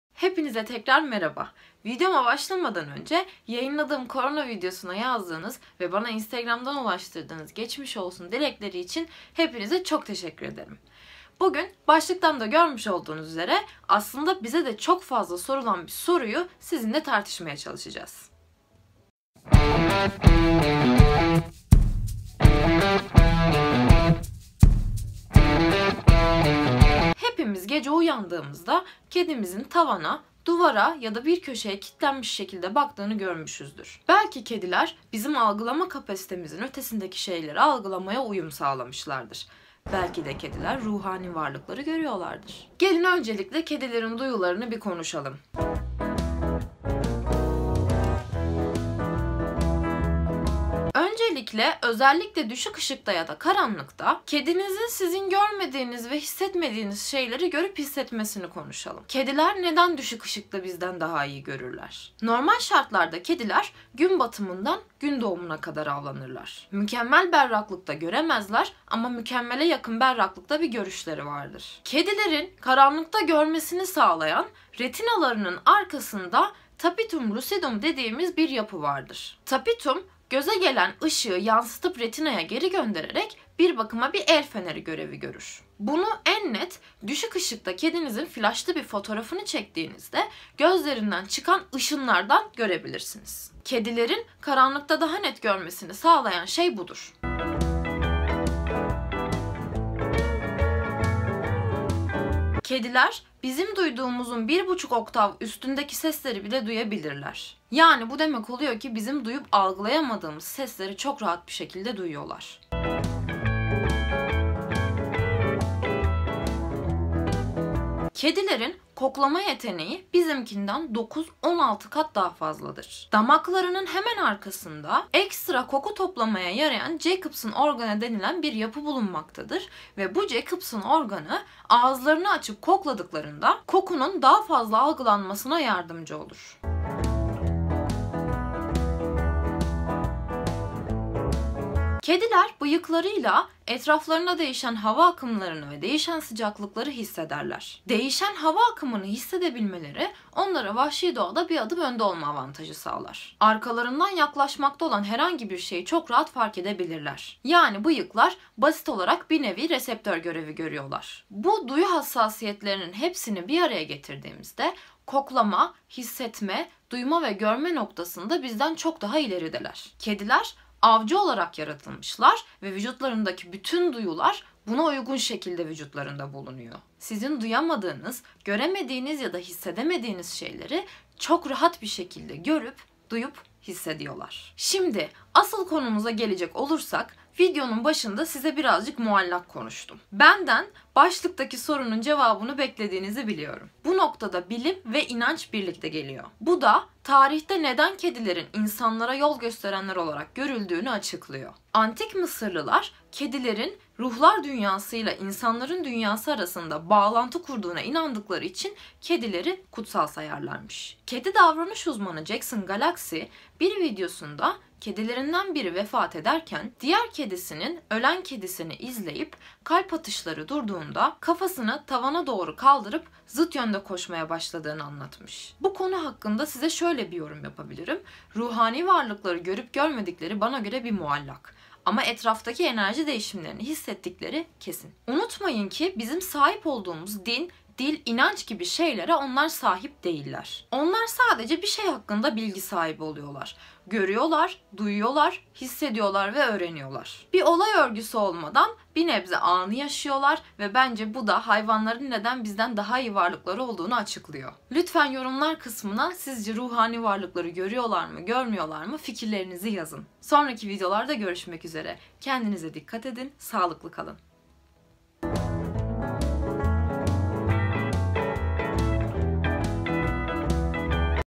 UZAYLILAR | Kediler Cinleri / Ruhları Görebilir Mi ?
Kediler belki üç harflileri görüyor, belki de duvarlara bakma sebepleri duvarın diğer tarafındaki sesleri ya da duvardaki çatlaklardan ve borulardan gelen sesleri duymaları, sizce hangisi ?

Fikirlerinizi yorumlara yazın !